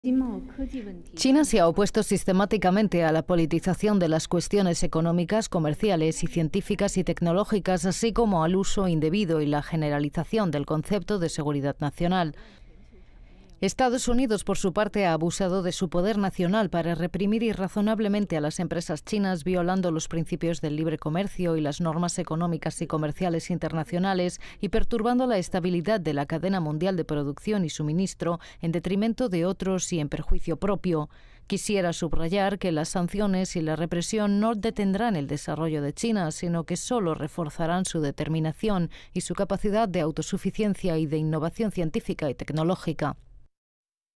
China advierte que las sanciones "no frenarán el desarrollo" del país
El Ministerio de Relaciones Exteriores de China ha reiterado en un comunicado de su portavoz que los esfuerzos de Estados Unidos para detener el desarrollo de China sólo fortalecerán su determinación de lograr la autosuficiencia. La cancillería china responde así a una declaración de Washington en la que asegura estar investigando el chip empleado en el último teléfono móvil de Huawei.